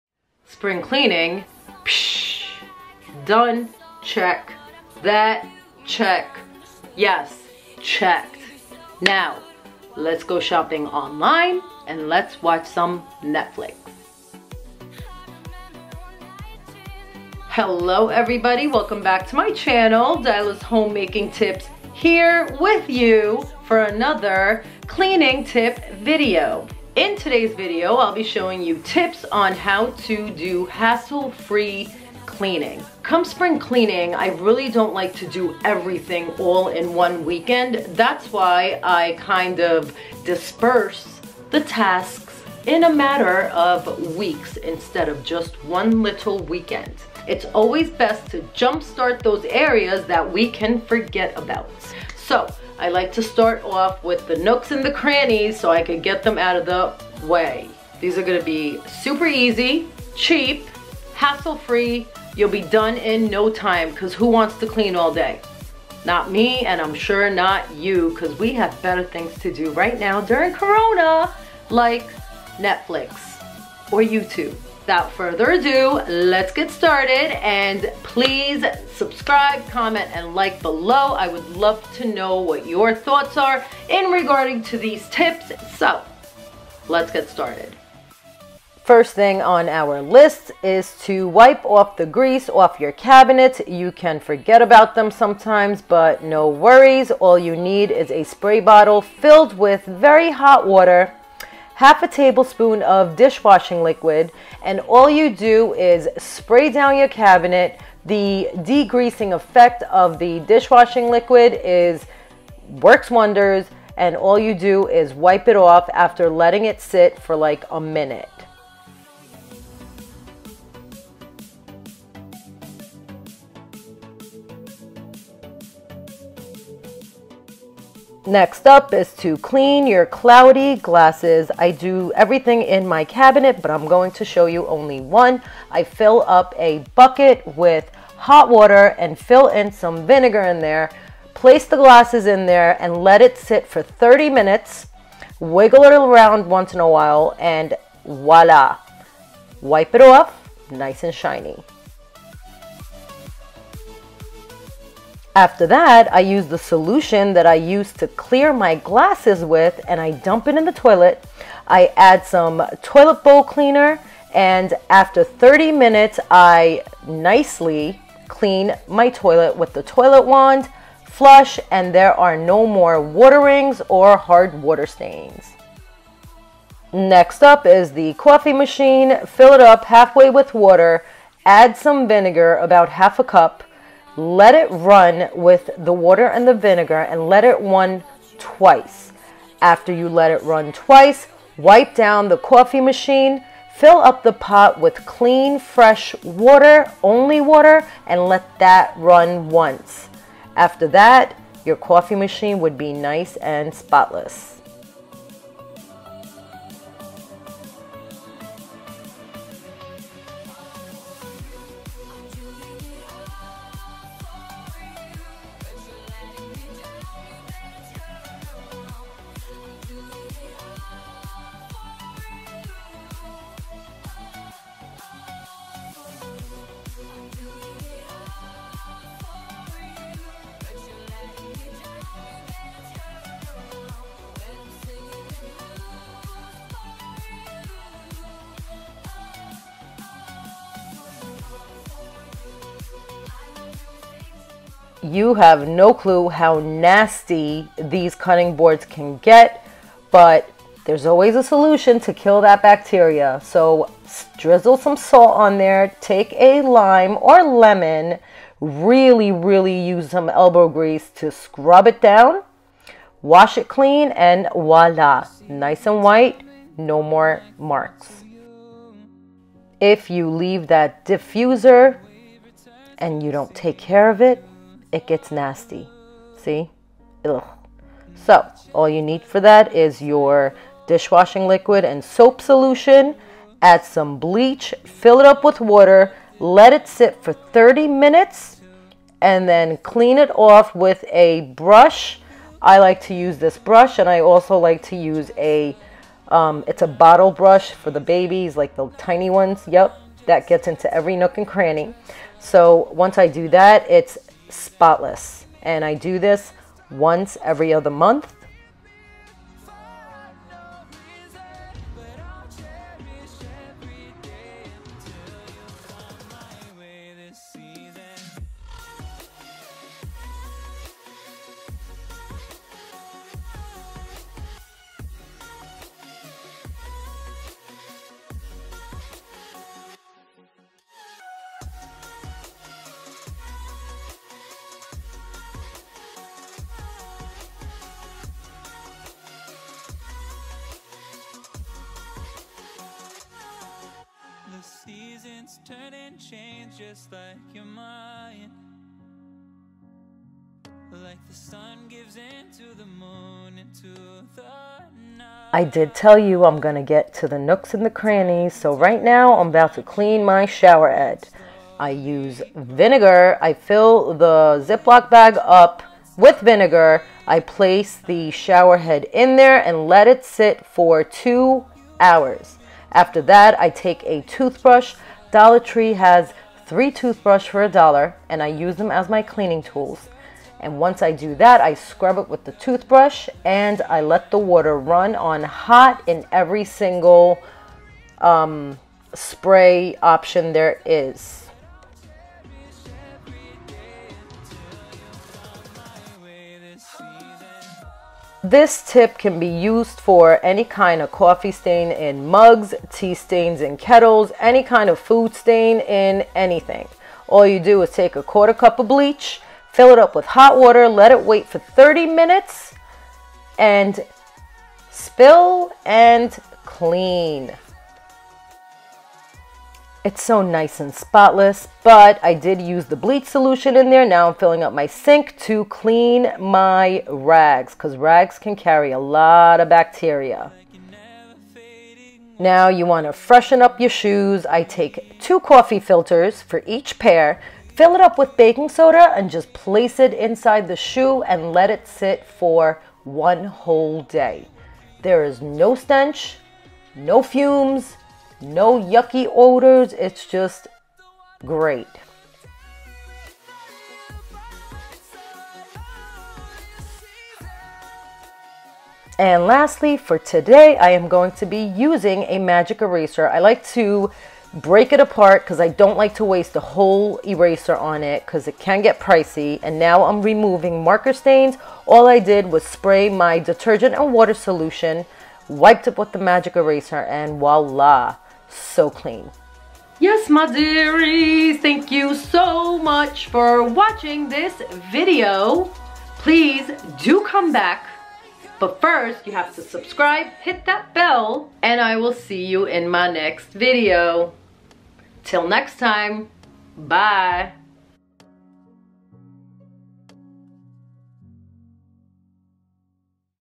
SPRING CLEANING HEAD START // Cleaning tips on detailed hassle free cleaning!!
HI FRIENDS
Today I am going to share my cleaning tips when starting my spring cleaning process.

I will show you hassle free, quick, and detailed cleaning tips to do in your home so you can make time for YOU